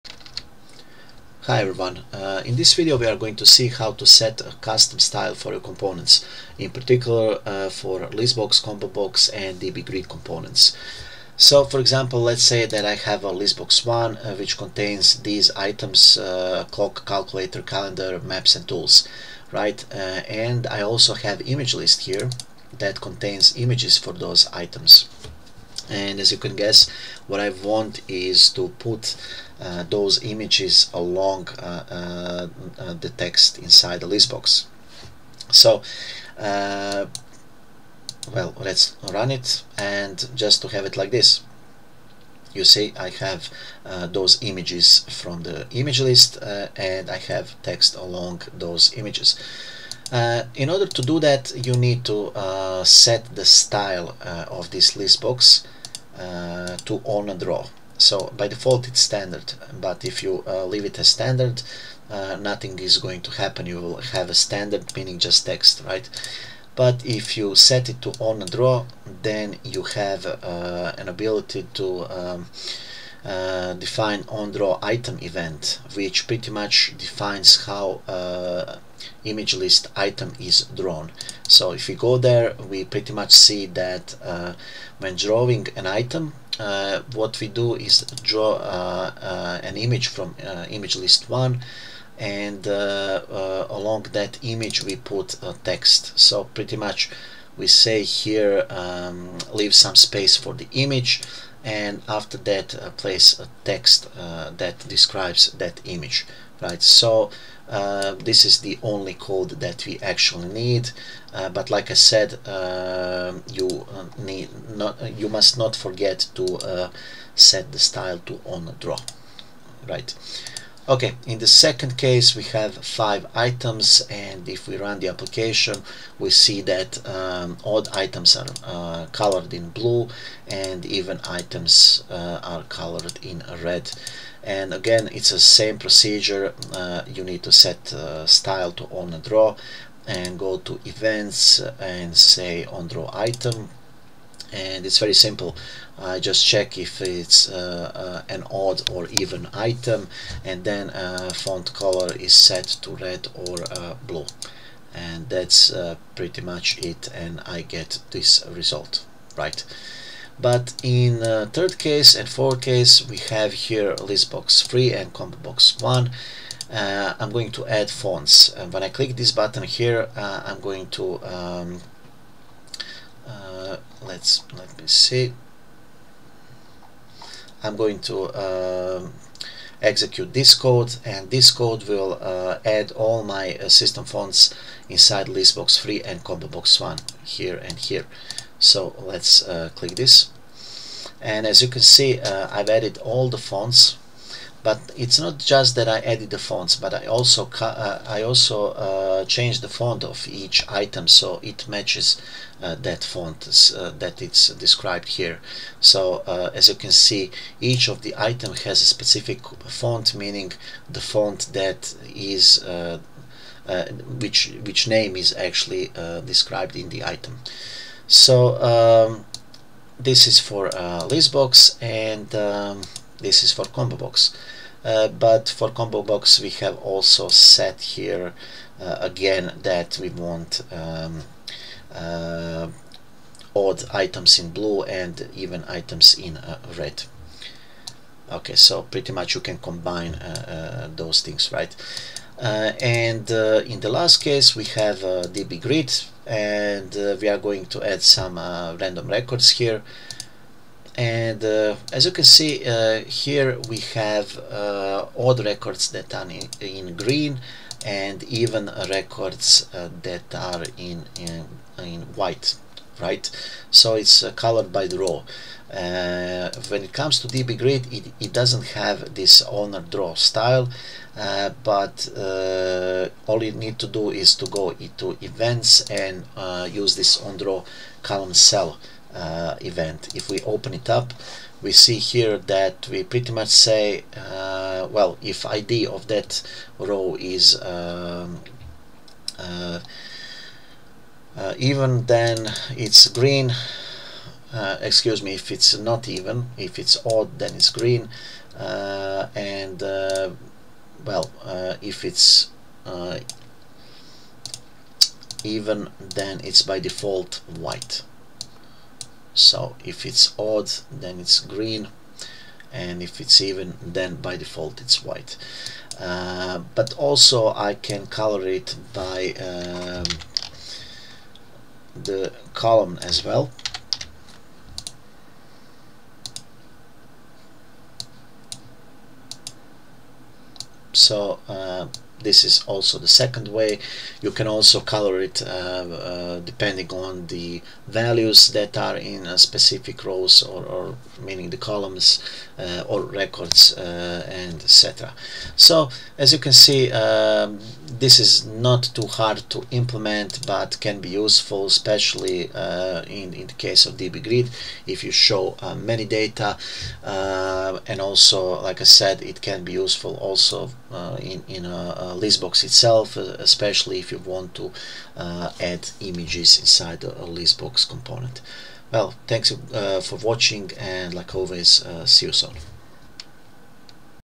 C++ Builder - Customizing ListBox, ComboBox and DBGrid Components
How to customizing items in ListBox, ComboBox and DBGrid components?